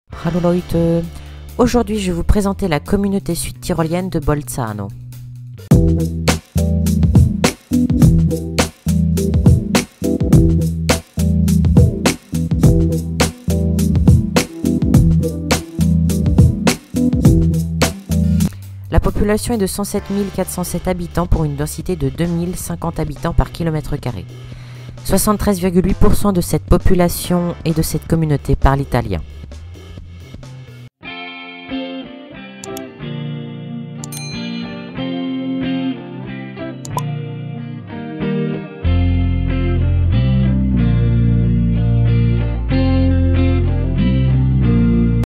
Communauté #1 : Bolzano
Cette vidéo vous informe sur la communauté sud tyrolienne de Bolzano.
Mon logiciel de montage (Meine Montage-Software) : Shotcut